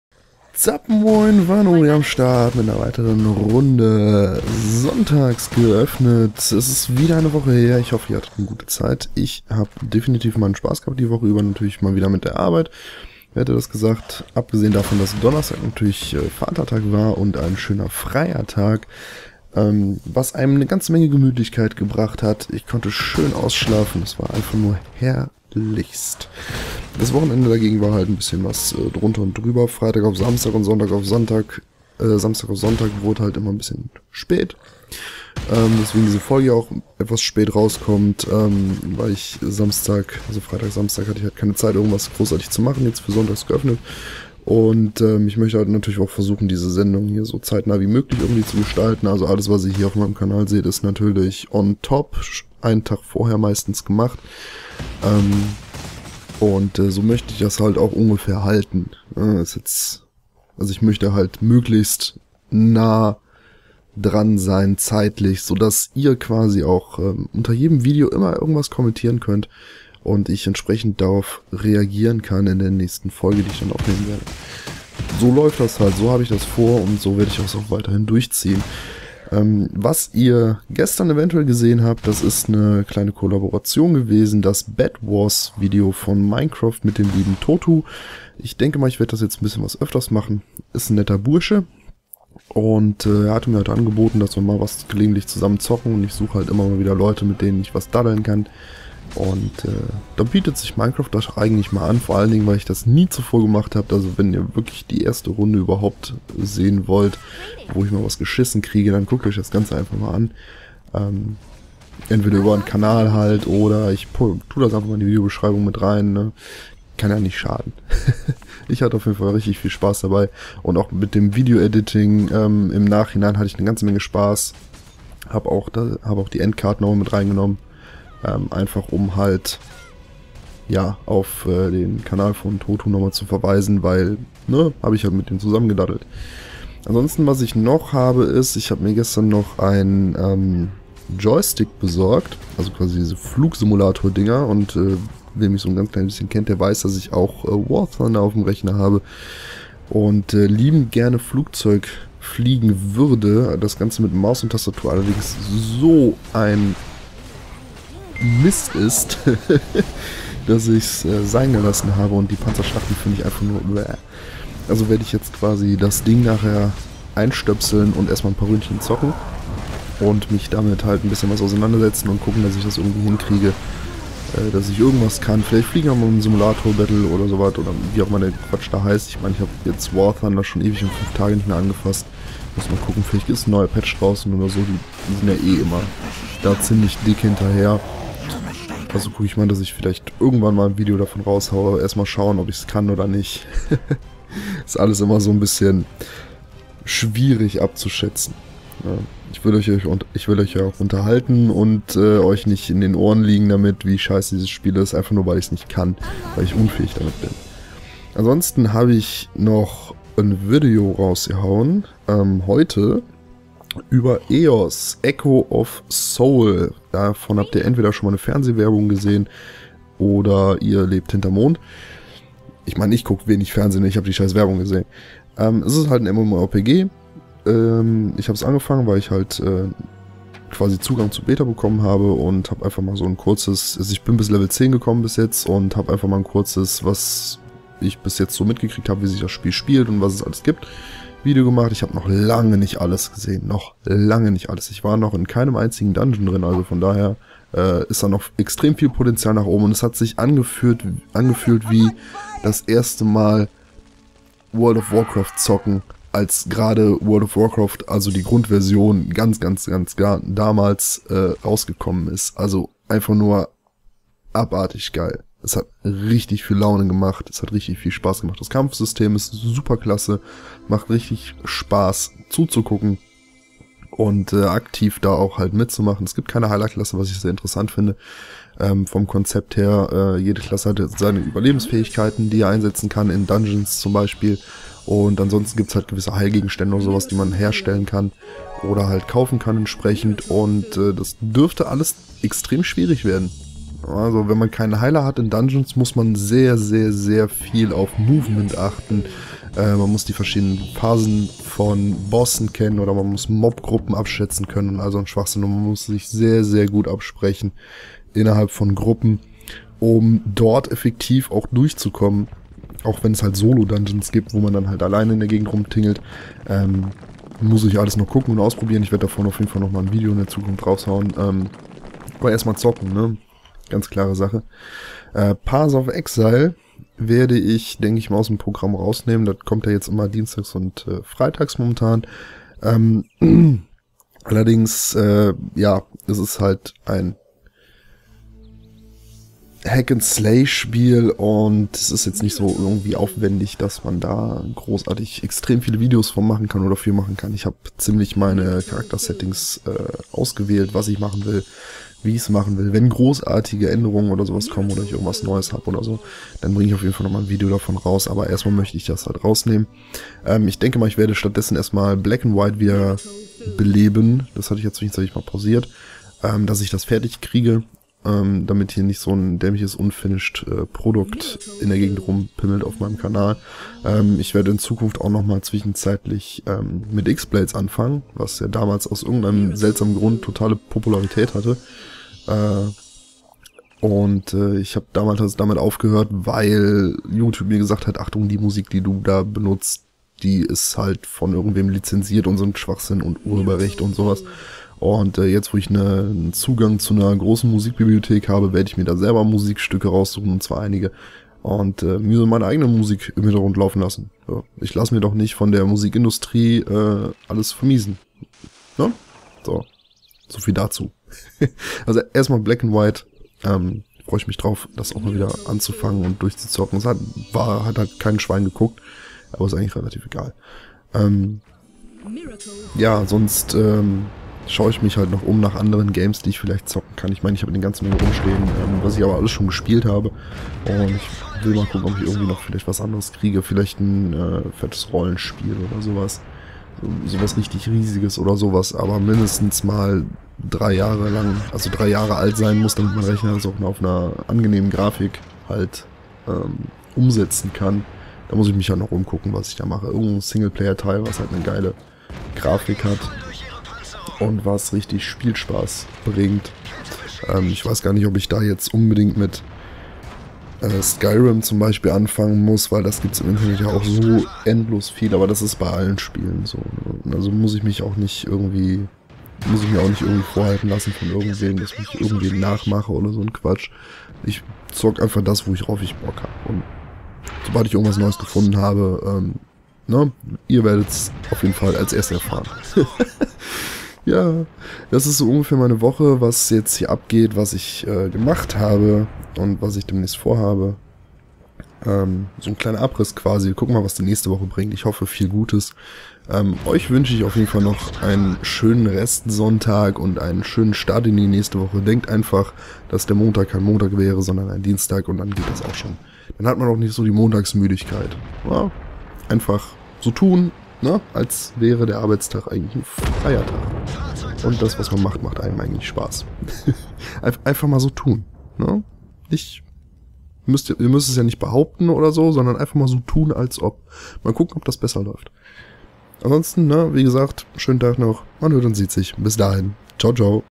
Sonntags Geöffnet ♥ Wochenrückblick | Folge 11
Endlich habe ich einen Joystick für Warthunder! Echo of Soul ist ein geiles MMO Mit dem lieben ToTuuu mache ich in Zukunft hoffentlich noch viele Videos und Path of Exile fliegt und Black and White kehrt dafür zurück.